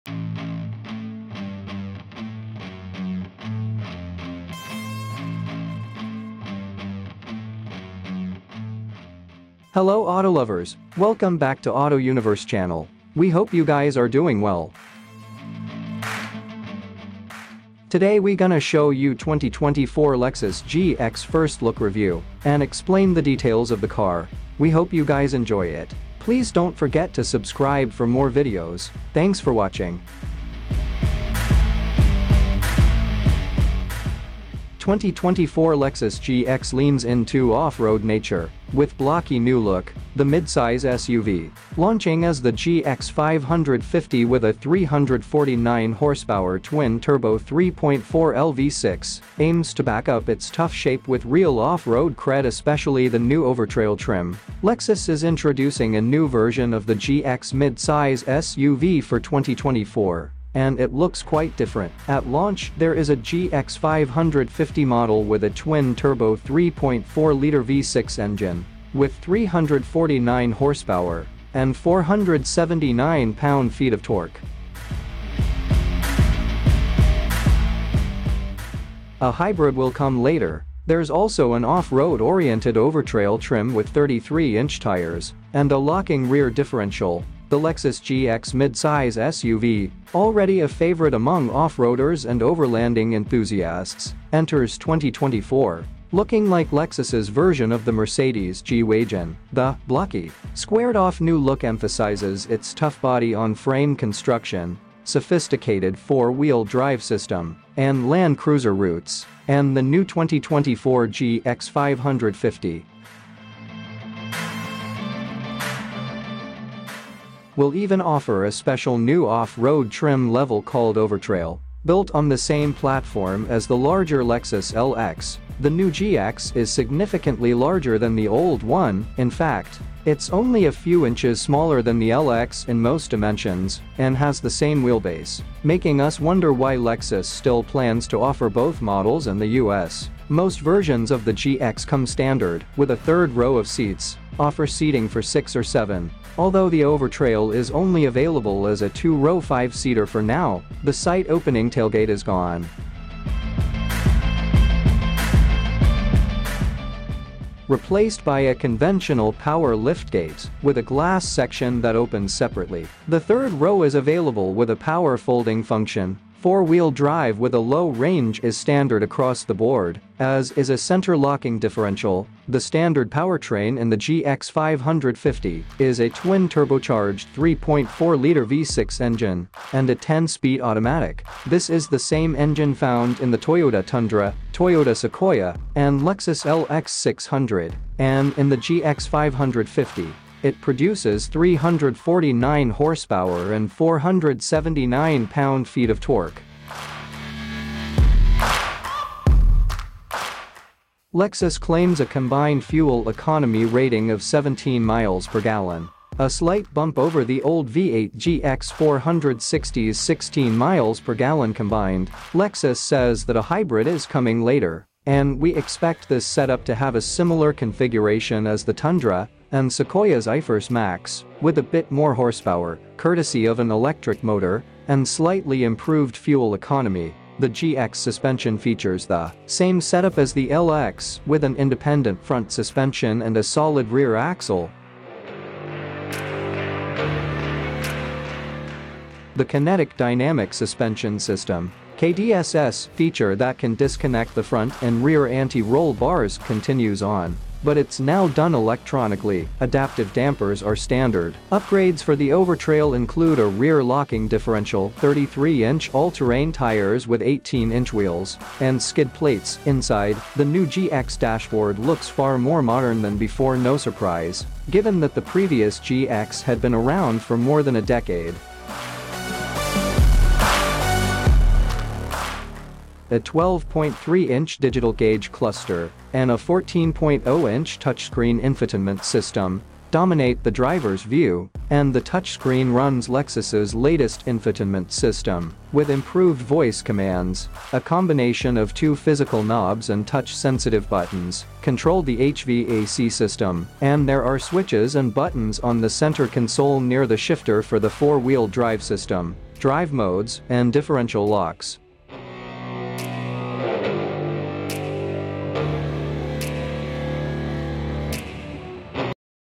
2024 Lexus GX already a favorite among off-roaders and overlanding enthusiasts
2024 lexus GX already a favorite among off-roaders and overlanding enthusiasts @QUOTESUNIVERSE88